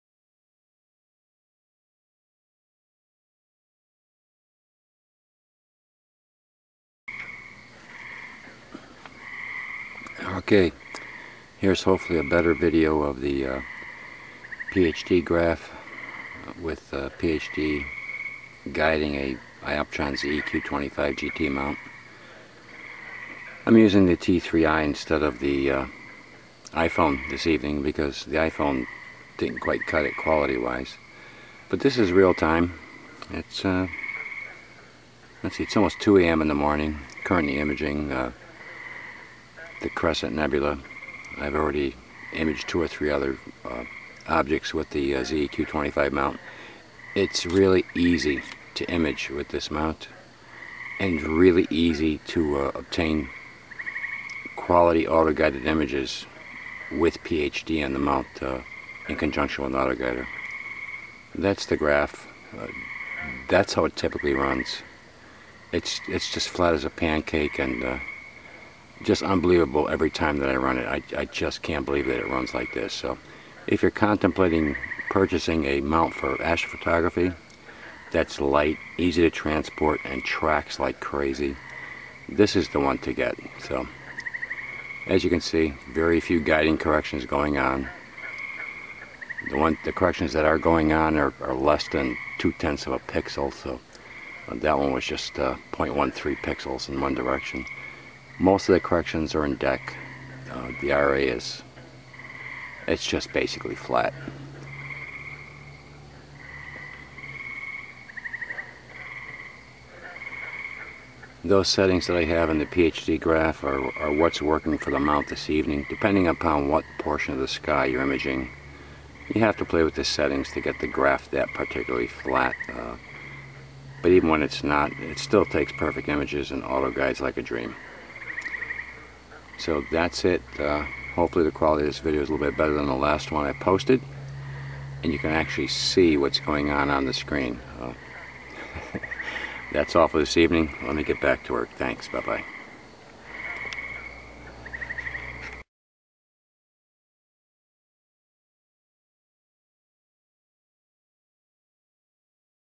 June 23, 2013 Autoguiding the ZEQ25
Just a short video of real time guiding with a iOtpron ZEQ25 and PHD.